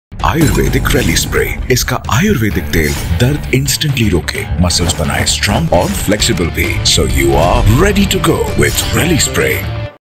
Relispray - Instant Pain Relief Spray - Hindi
Natural Relispray is trusted by millions of athletes and coaches for its instant pain relief action. It has the highest concentration of 7 Ayurvedic ingredients – Eucalyptus oil, Cinnamon Oil, Clove Oil, Turpentine Oil , Menthol, Camphor and Wintergreen Oil.  Natural Relispray’s direct spray action unlike balms requires no massage and with every spray the healing ingredients go deep inside the skin. It also helps in strengthening and healing muscles without any side effect.